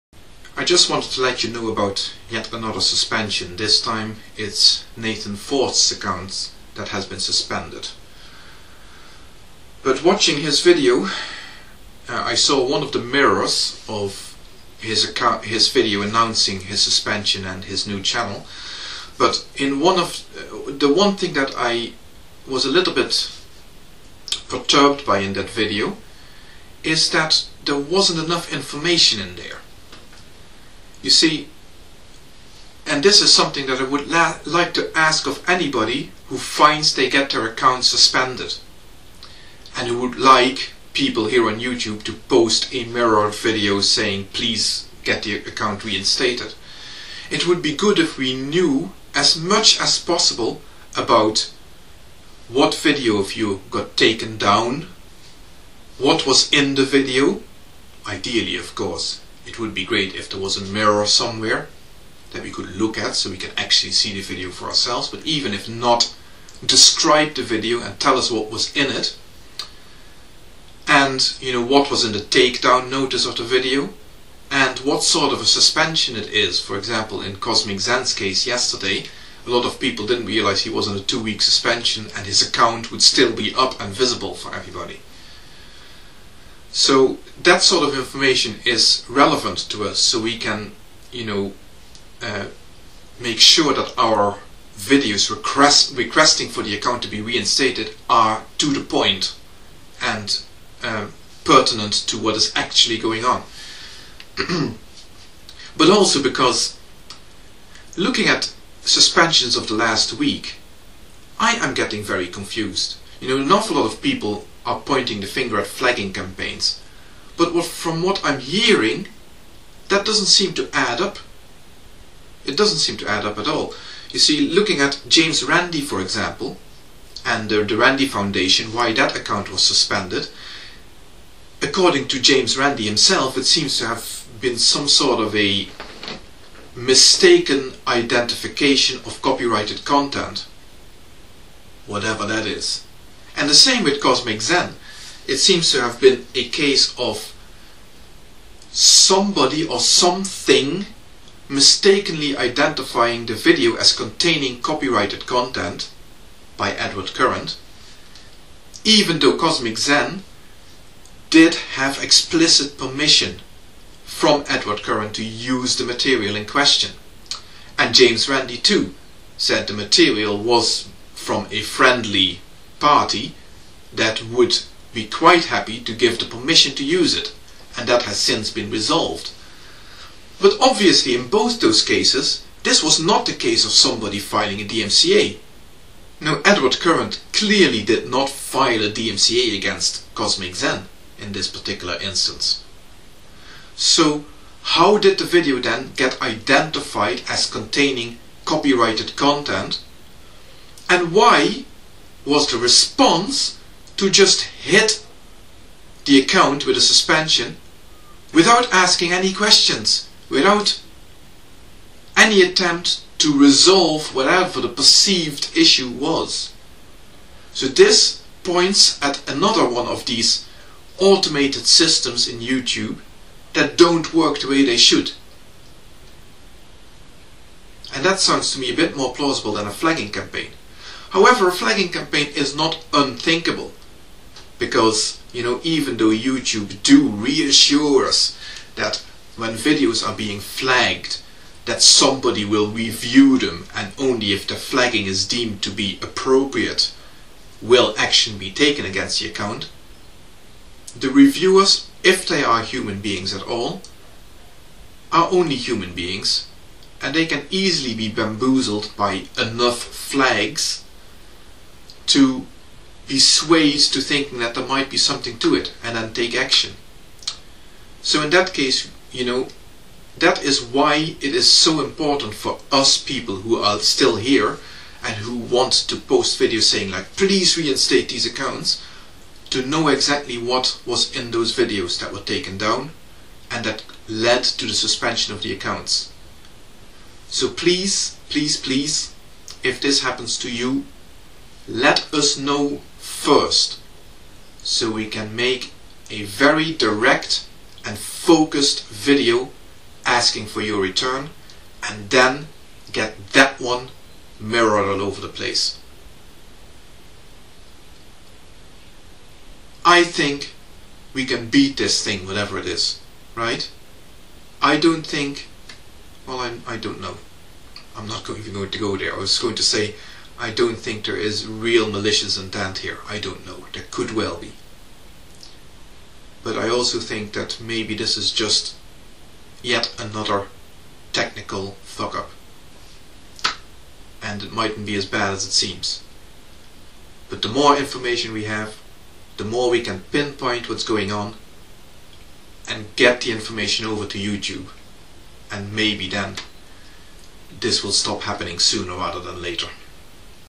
in gaffa?
what's with all the suspensions? Also a request of all suspended users to provide us with as much information as humanly possible.